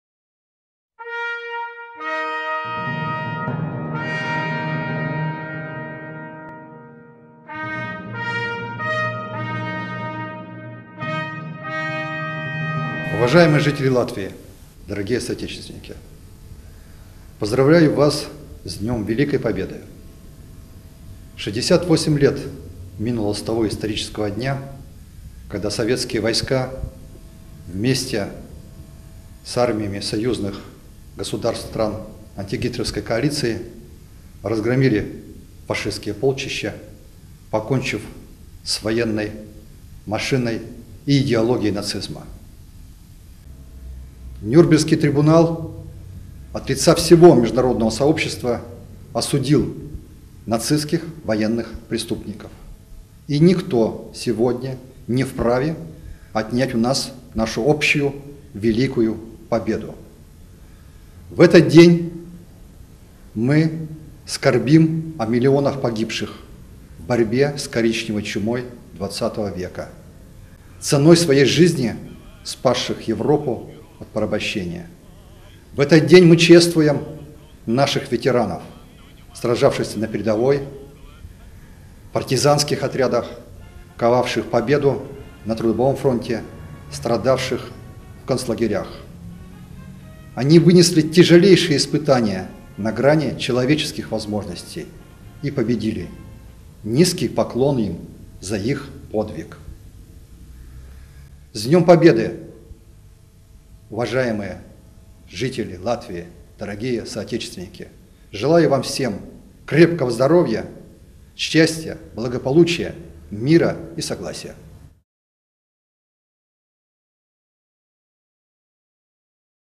Поздравление Посла России с Днём Победы
Поздравление Посла Российской Федерации в Латвийской Республике Вешнякова А.А. с Днём Победы